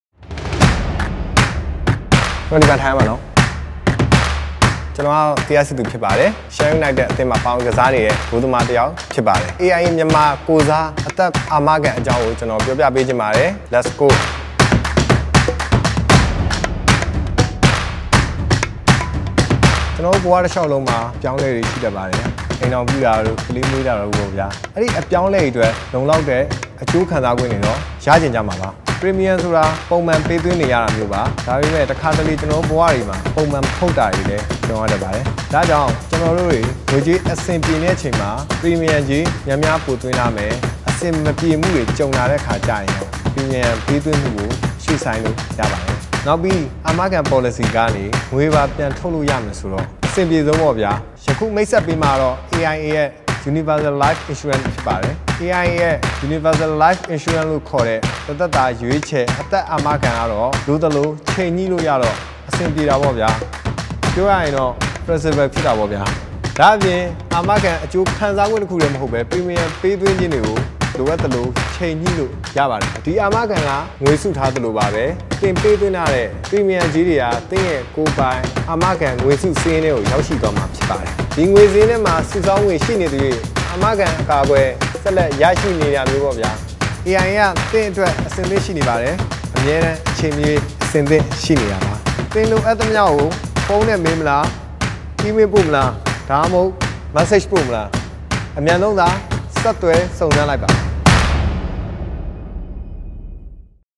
AIA Myanmar x Myanmar National League : Ep 6
Shan United's Thiha Si Thu knows all about twists and turns and keeping your eye on the ball. So there's no one better to help close out our series on insurance basics with an explanation of Universal Life-- the product that goes in every direction you do.

We hope these videos have made insurance easier to navigate and thank all the players for participating! Let us know what you think!